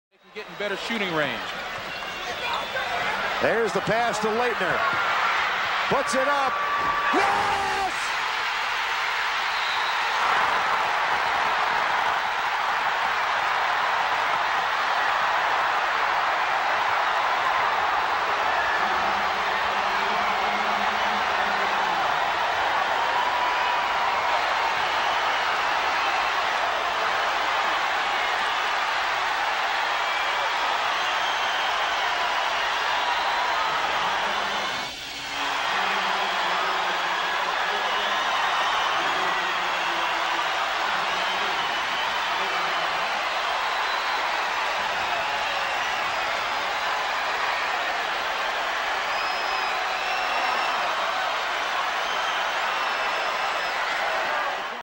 Duke vs. Kentucky 1992
The is the final shot in the Duke vs. Kentucky match-up in 1992